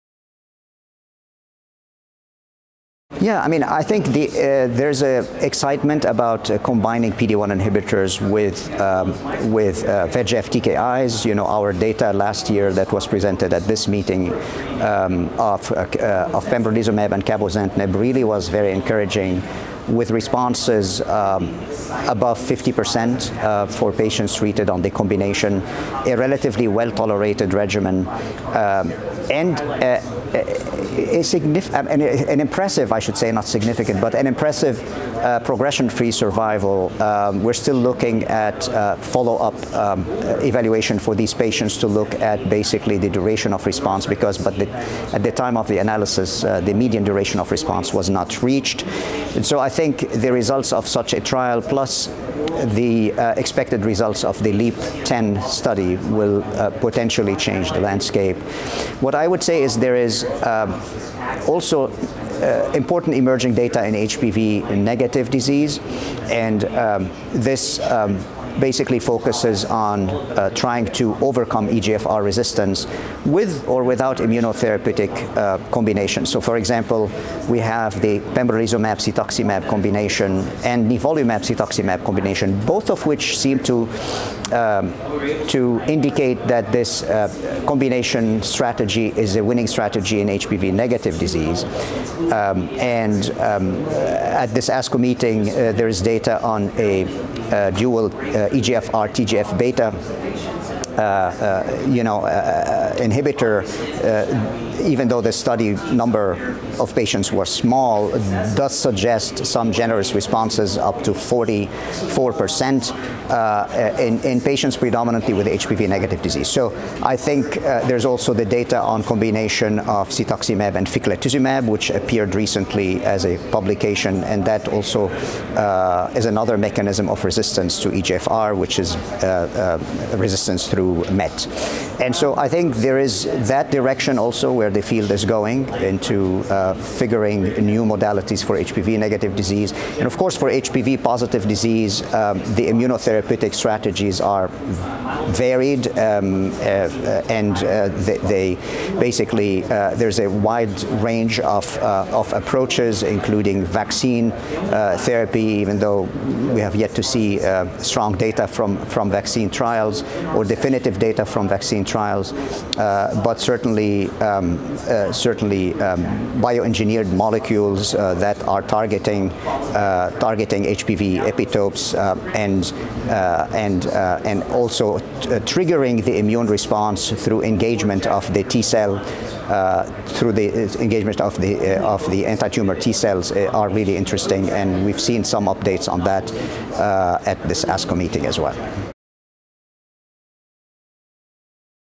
Novel immunotherapy approaches in metastatic head and neck cancer
Nabil Saba, MD, FACP, Winship Cancer Institute of Emory University, Atlanta, GA, gives an overview of developments in utilizing immunotherapy for patients with metastatic head and neck cancer. The combination of PD-1 and VEGF inhibitors such as pembrolizumab and cabozantinib is a promising strategy, and findings from the LEAP-010 trial (NCT04199104) of pembrolizumab with lenvatinib further demonstrated the efficacy of this combination. Cetuximab with ficlatuzumab has additionally been assessed in a Phase II trial (NCT03422536) in patients with HPV-negative head and neck cancer. This interview took place at the American Society of Clinical Oncology (ASCO) 2023 Annual Congress in Chicago, IL.